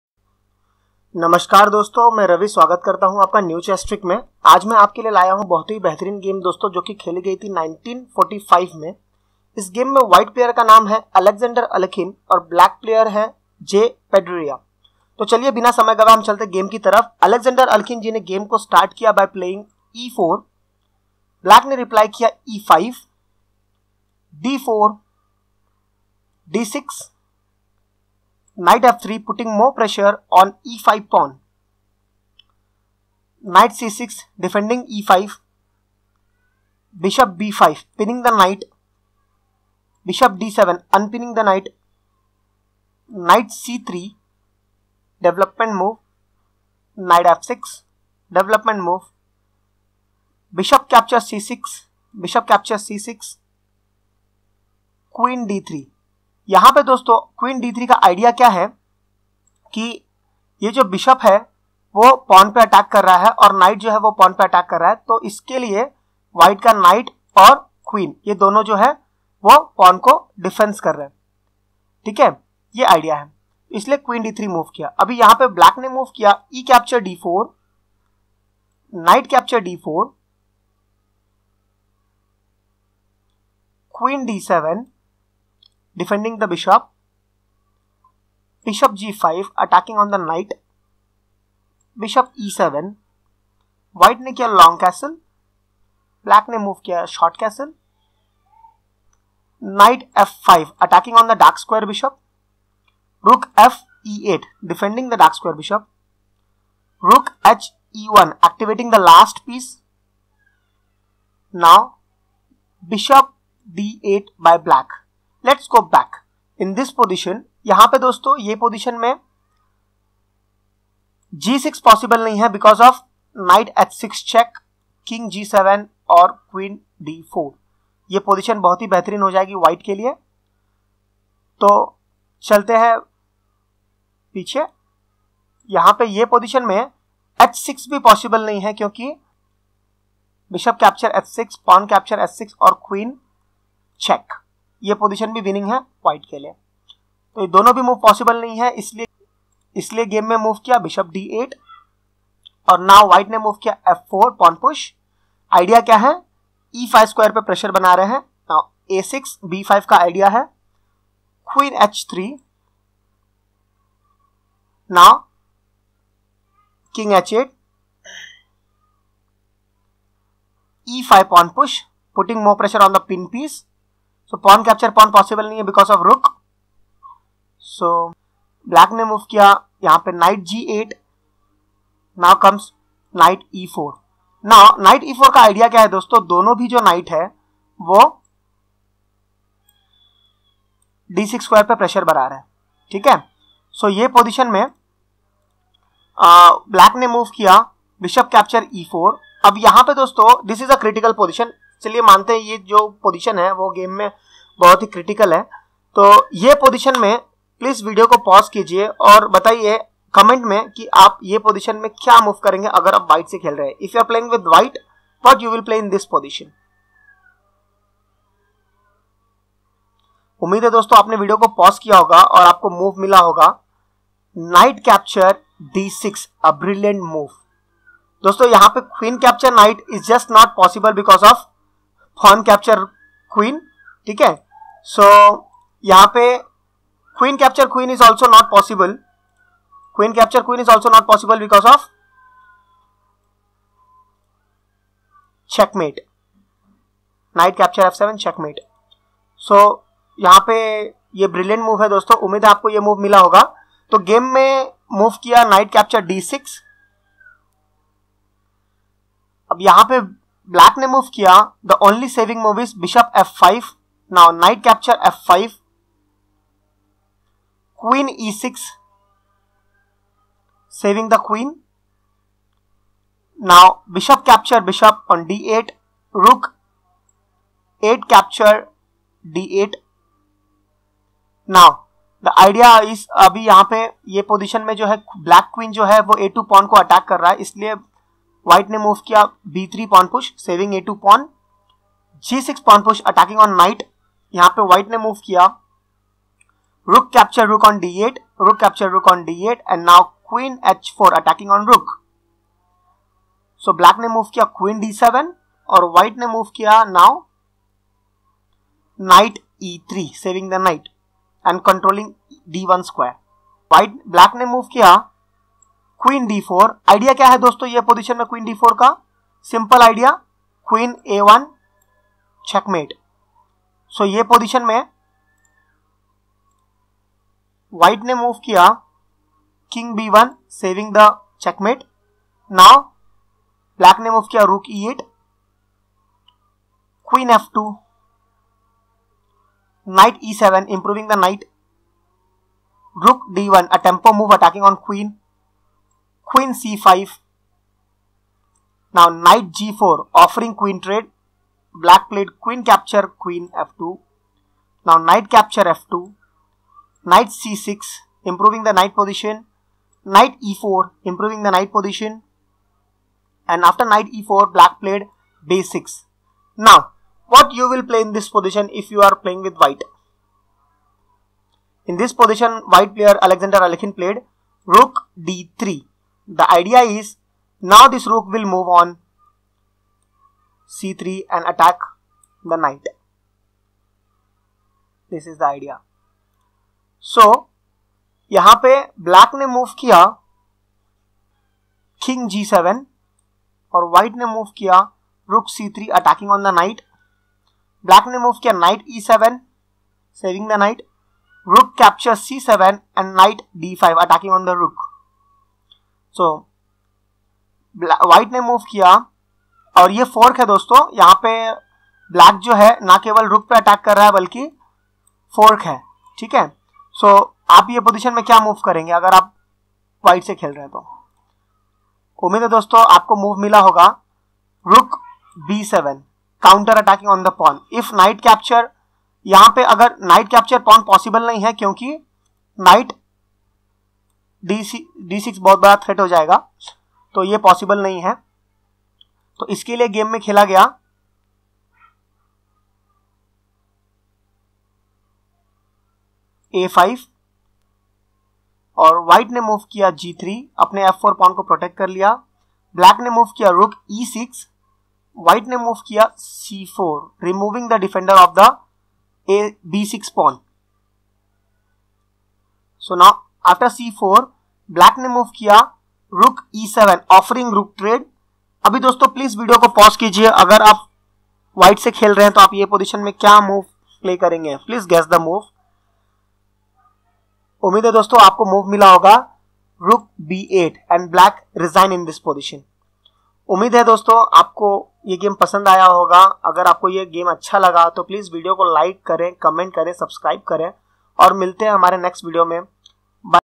NEW CHESS TRICKS FOR WHITE in Ruy Lopez Alexander Alekhine Game 2
LEARN FROM THE MASTERS GAME
Ruy Lopez C62
Alexander Alekhine VS J Pedreira
1945